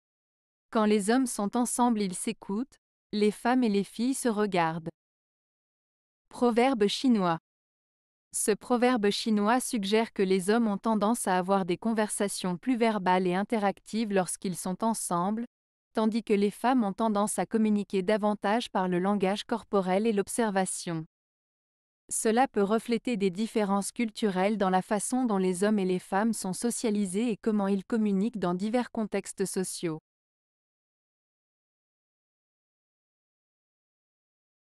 Quand les hommes sont ensemble ils s'écoutent, les femmes et les filles se regardent.
Ce proverbe chinois suggère que les hommes ont tendance à avoir des conversations plus verbales et interactives lorsqu'ils sont ensemble, tandis que les femmes ont tendance à communiquer davantage par le langage corporel et l'observation. Cela peut refléter des différences culturelles dans la façon dont les hommes et les femmes sont socialisés et comment ils communiquent dans divers contextes sociaux.